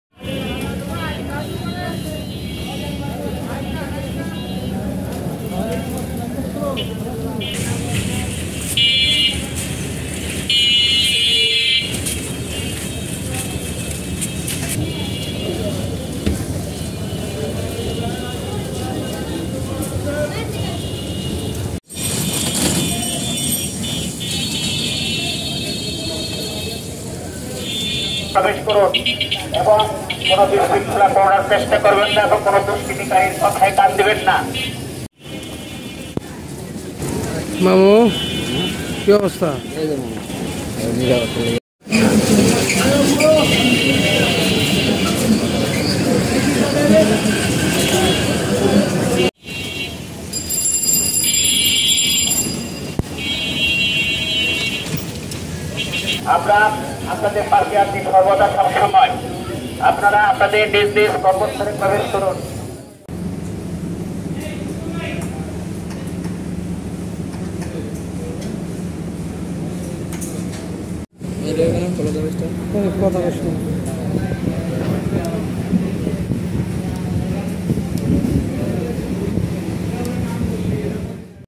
Ashulia Graments open Footage 08 08 1
শিল্পাঞ্চল আশুলিয়ায় চালু হয়েছে গার্মেন্টস ফ্যাক্টরি। বৃহৎস্পতিবার(৮আগস্ট) সকাল থেকে আশুলিয়ার বিভিন্ন এলাকার পোশাক শ্রমিকদের কাজে যোগ দিয়েছেন।